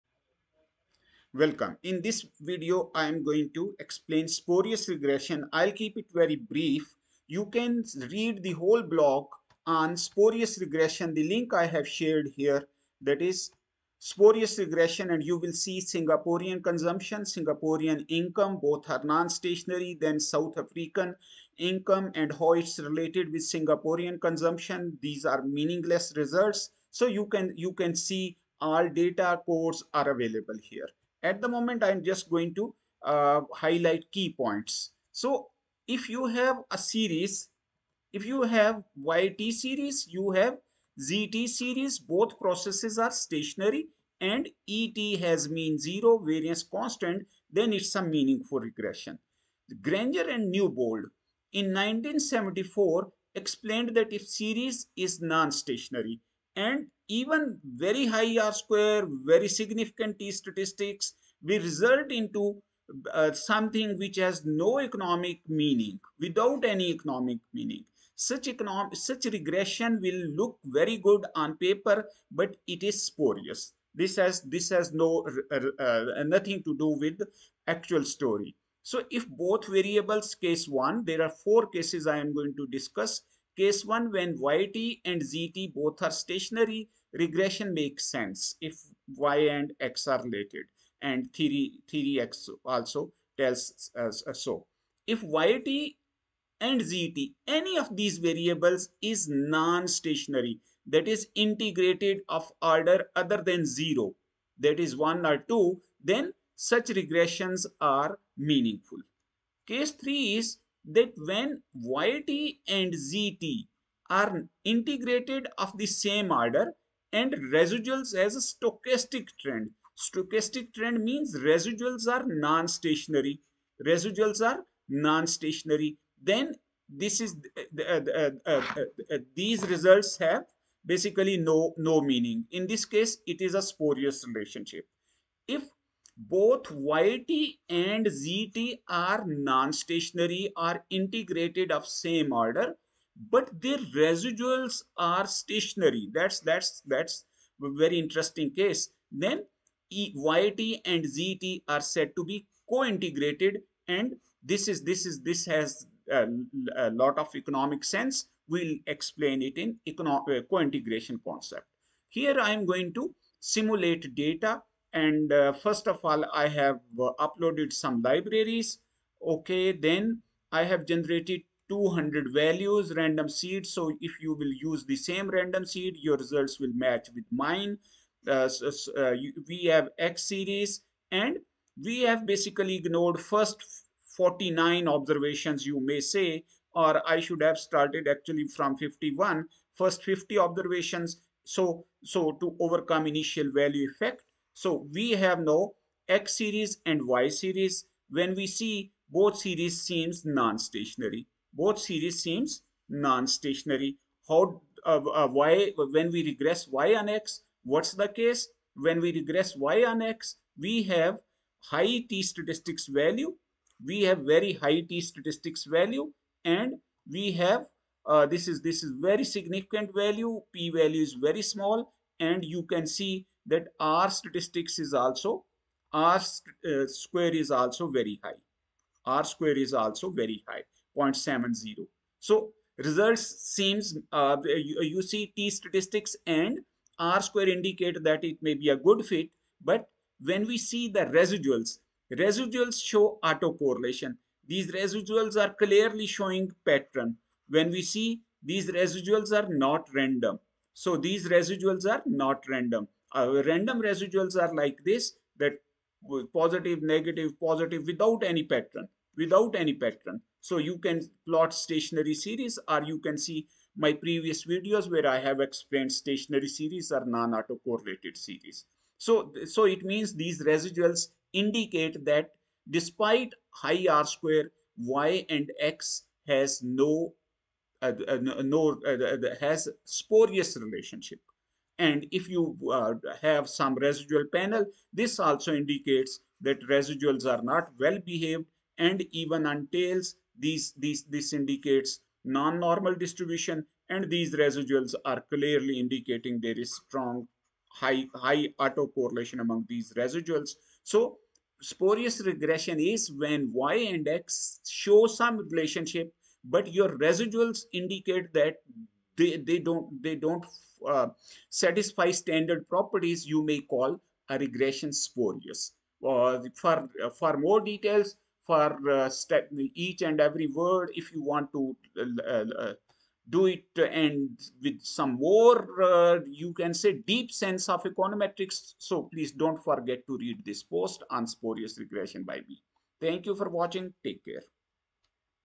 Spurious Regression in R
In case 2 when x or y is nonstationary, regression is meaningless (a minor clarification)